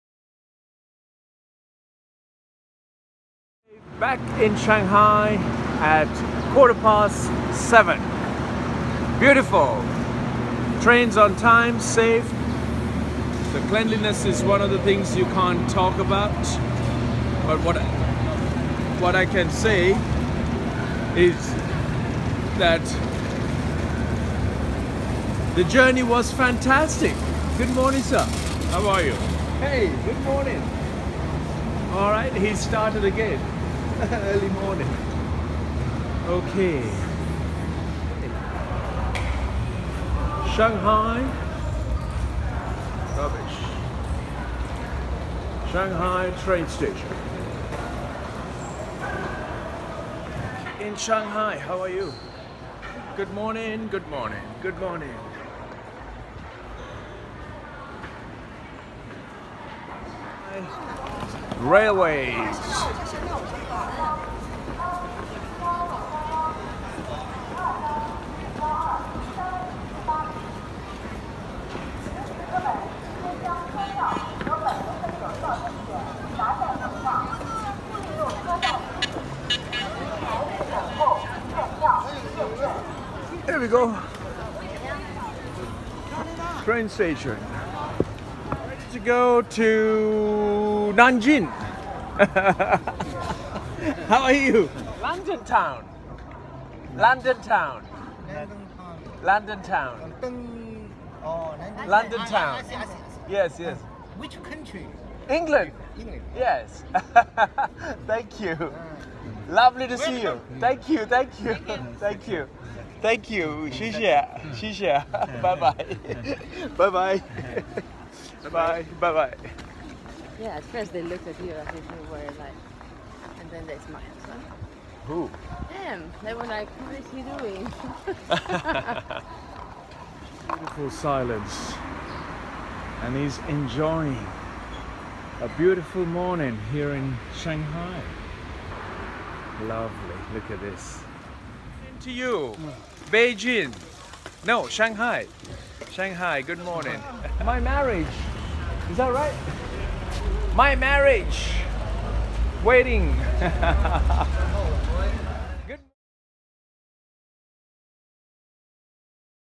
Arriving in Shanghai Railways Station
Arriving at Shanghai Railways Station, and getting connected to the Metro system.

The Station is large like a Airport size Terminal serving trains towards west and northwest.

I am always fascinated to walk through Train stations as people who are travelling from all parts of China are always outside of the station.